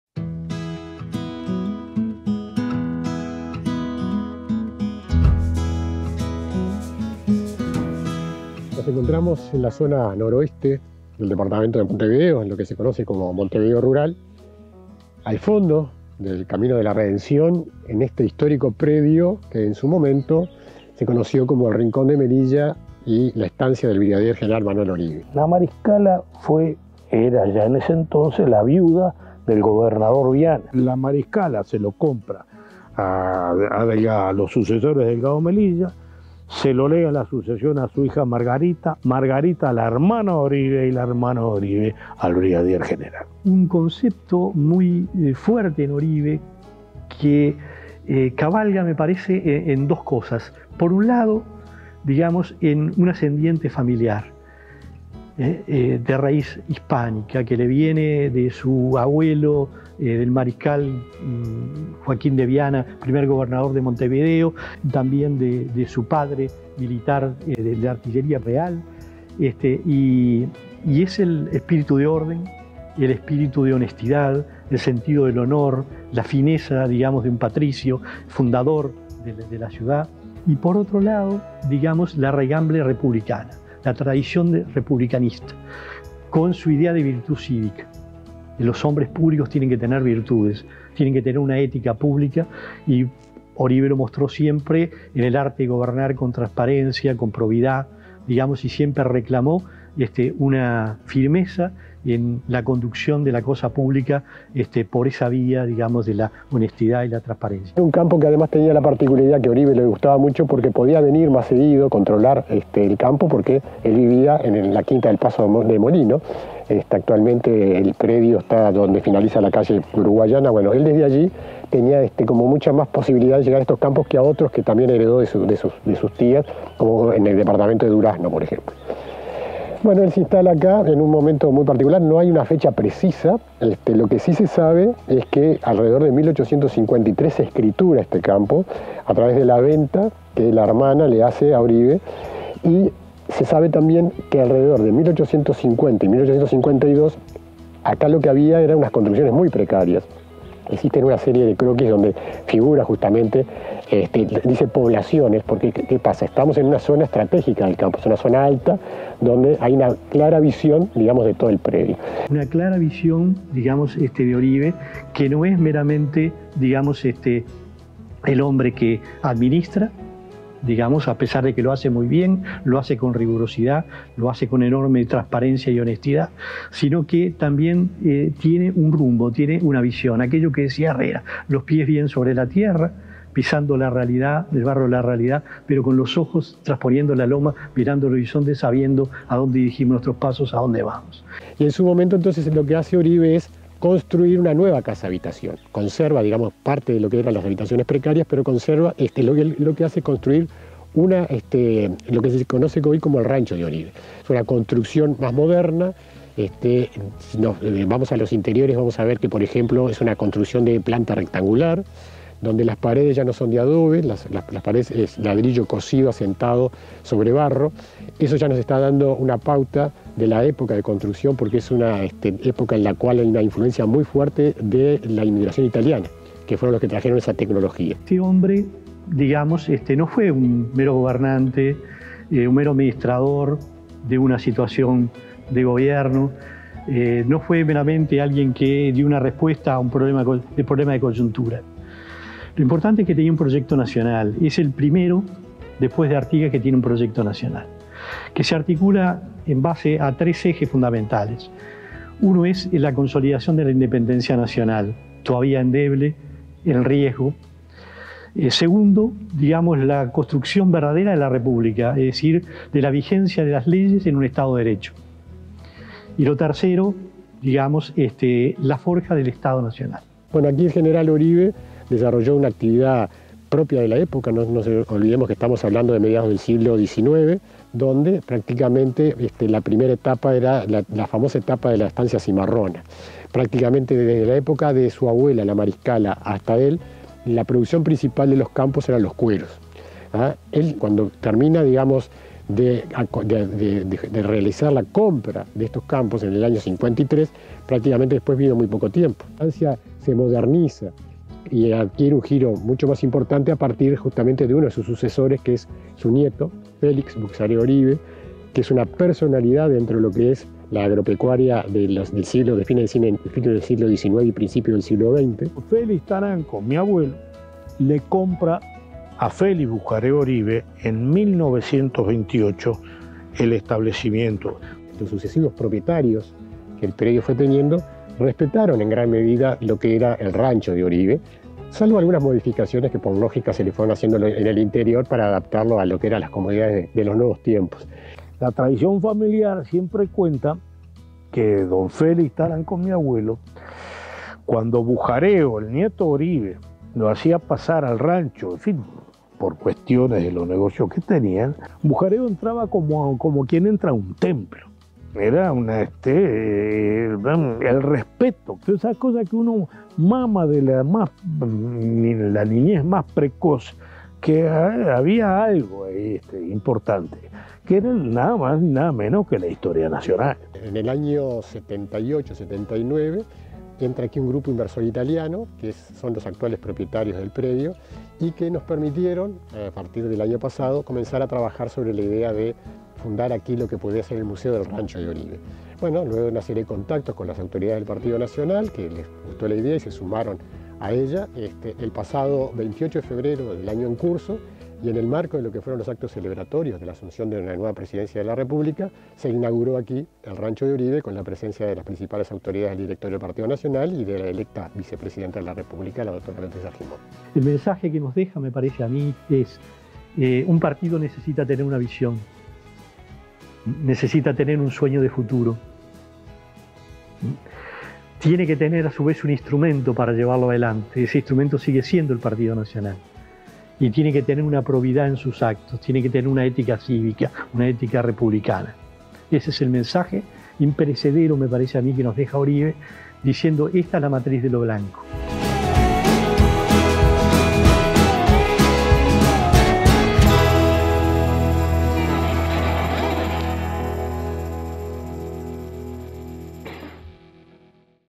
Museo Rancho de Oribe y su historia (documental)
Relatan sobre la personalidad de Oribe y la historia del Museo Rancho de Oribe, José Ortiz de Taranco, el Ing. Agr. Mario R. Correa y Lic. Daniel Corbo